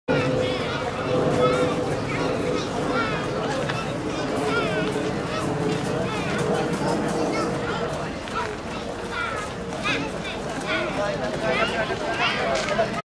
Nunhead Cemetery Open Day 2011 - our stand
Browsing our bookstall on 21 May 2011